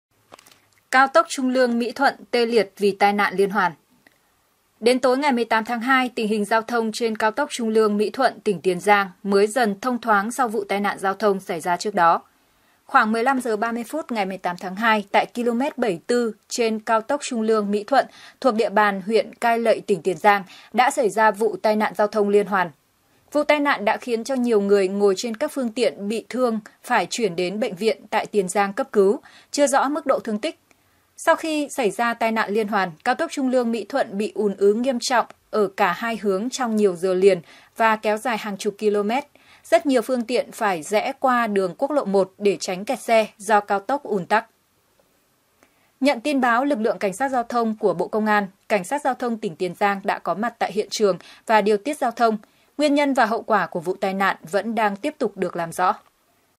Kẹt xe kéo dài trên cao tốc Trung Lương - Mỹ Thuận sau tai nạn liên hoàn | THVN
Một vụ tai nạn liên hoàn 7 xe đã xảy ra trên cao tốc Trung Lương - Mỹ Thuận khiến nhiều xe hư hỏng nặng, kẹt xe kéo dài hướng về Trung Lương để đi TP.HCM.